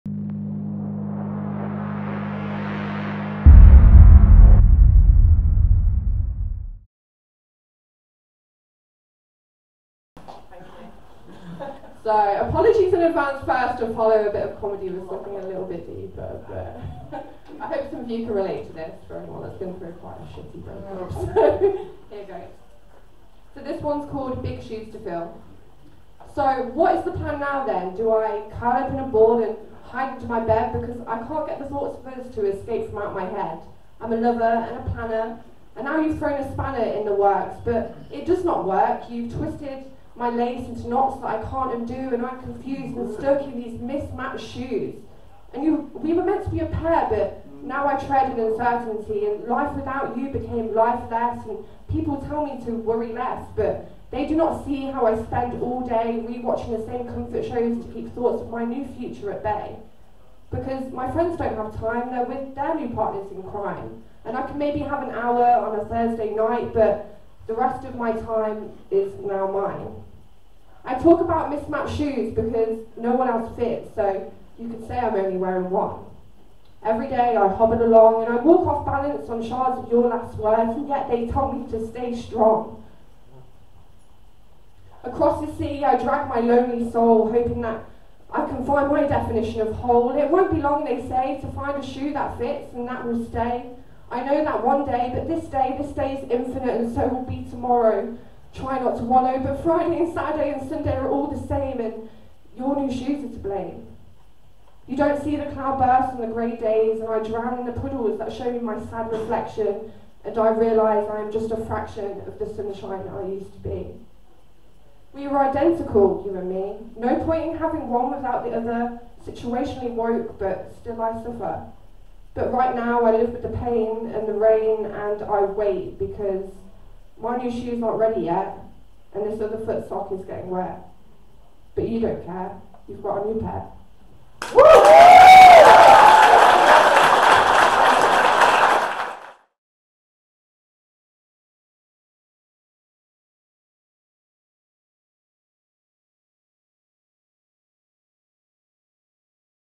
05 Mel Slam Round 1 @ Some-Antics 17-02-2023
Some-Antics is a spoken word event that takes place monthly at Grays Coffee Shop and Kitchen, Leicester. It has three sections- A featured act, spoken word by local poets and a poetry slam. If you would like to get involved please let us know on the Facebook event page